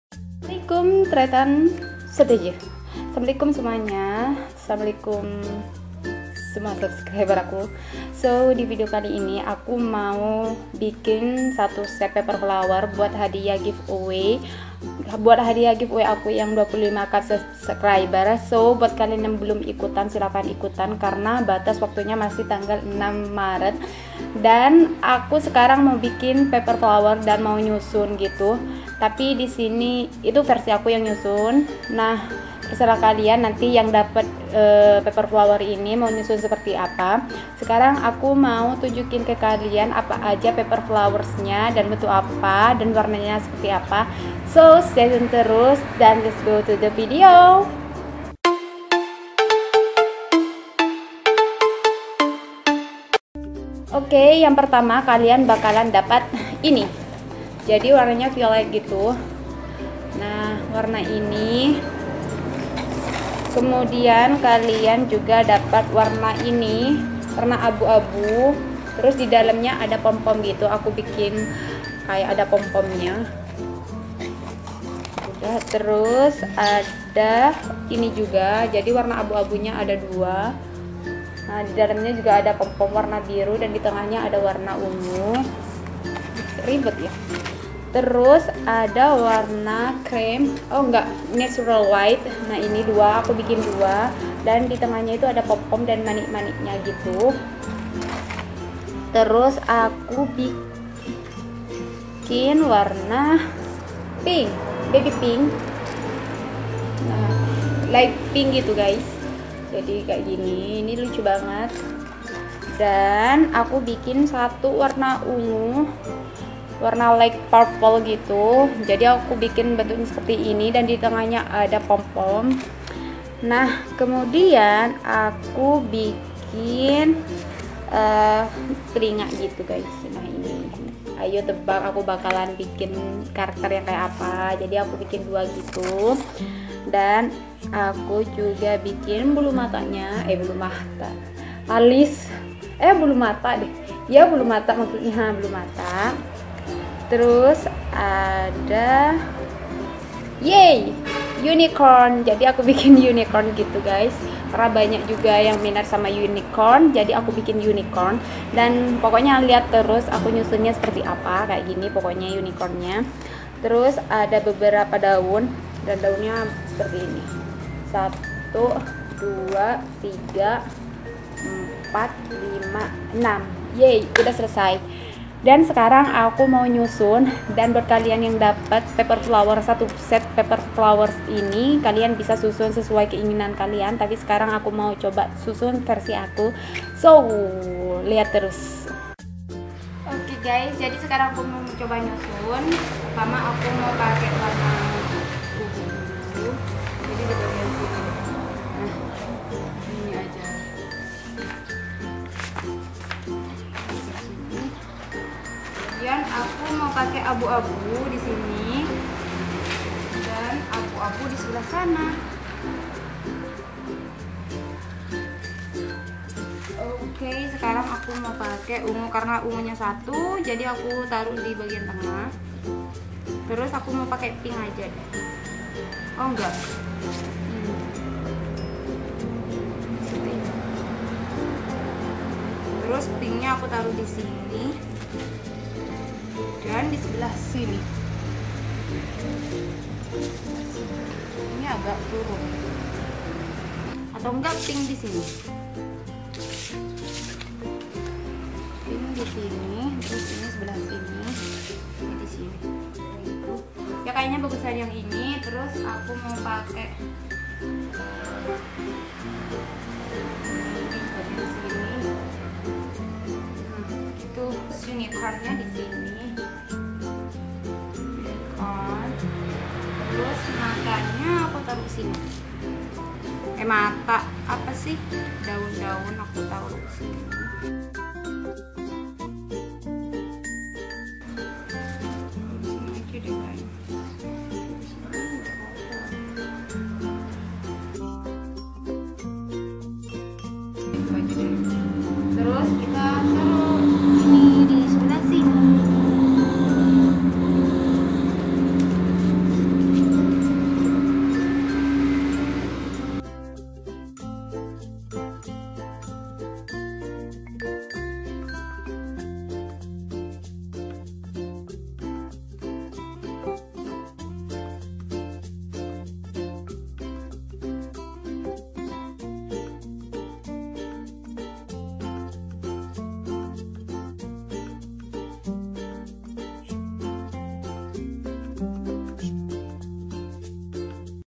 BIKIN 1 SET PAPER FLOWERS UNICORN UNTUK SUBSCRIBER by Farida FR
Jadi paper FLOWERS INI KHUSUS BUAT SUBSCRIBER YANG MENANG DI GIVEAWAY AKU YANG KE 3

 Yang mau order bisa langsunh klik link dibawah

Alat dan bahan membuat paper flowers:
1. Kertas jasmine

2. Pola paper lfowers

3. Cuttingmat n pen cutter

4. Mesin pemotong kertas

Tonton Juga